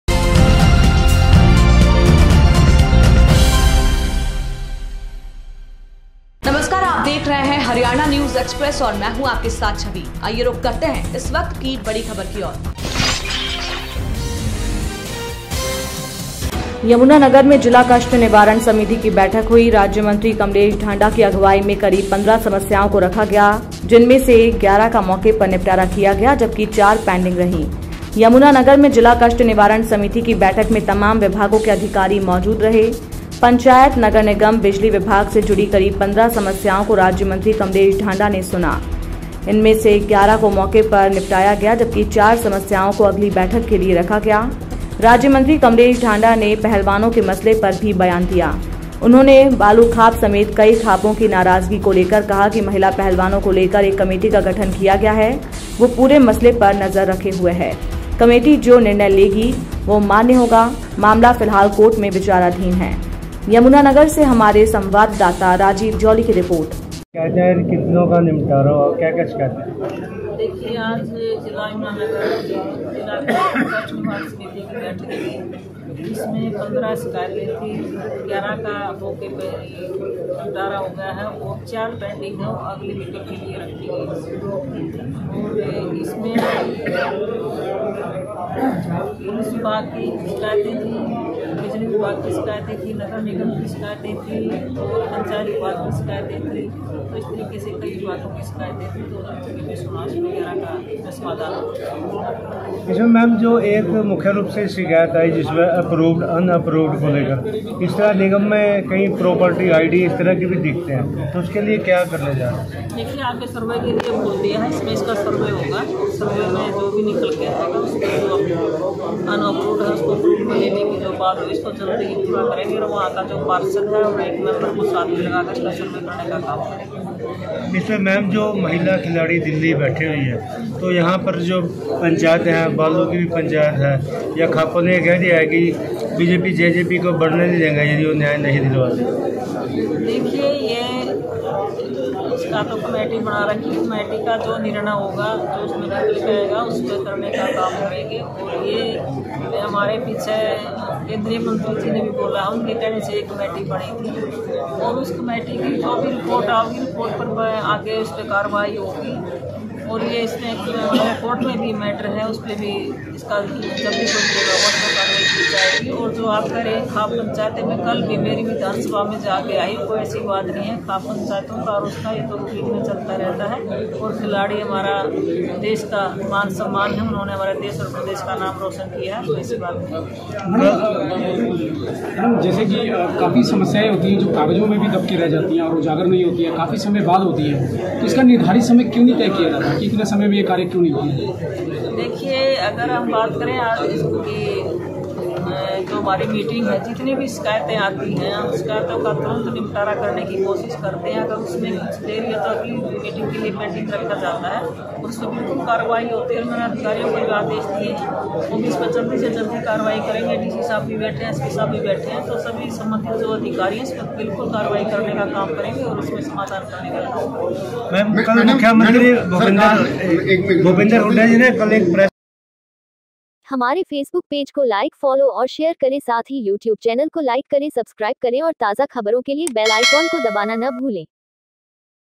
यमुनानगर : कमलेश ढांडा ने जिला कष्ट निवारण समिति की बैठक में सुनीं 15 जनसमस्याएं
यमुनानगर में जिला कष्ट निवारण समिति की बैठक हुई। राज्यमंत्री कमलेश ढांडा की अगुवाई में करीब 15 समस्याओं को रखा गया, जिनमें से 11 का मौके पर निपटारा किया गया जबकि 4 पेंडिंग रही। यमुनानगर में जिला कष्ट निवारण समिति की बैठक में तमाम विभागों के अधिकारी मौजूद रहे। पंचायत, नगर निगम, बिजली विभाग से जुड़ी करीब 15 समस्याओं को राज्यमंत्री कमलेश ढांडा ने सुना। इनमें से 11 को मौके पर निपटाया गया जबकि 4 समस्याओं को अगली बैठक के लिए रखा गया। राज्यंमत्री कमलेश ढांडा ने पहलवानों के मसले पर भी बयान दिया। उन्होंने बालू खाप समेत कई खापों की नाराजगी को लेकर कहा कि महिला पहलवानों को लेकर एक कमेटी का गठन किया गया है वो पूरे मसले पर नजर रखे हुए है। कमेटी जो निर्णय लेगी वो मान्य होगा। मामला फिलहाल कोर्ट में विचाराधीन है।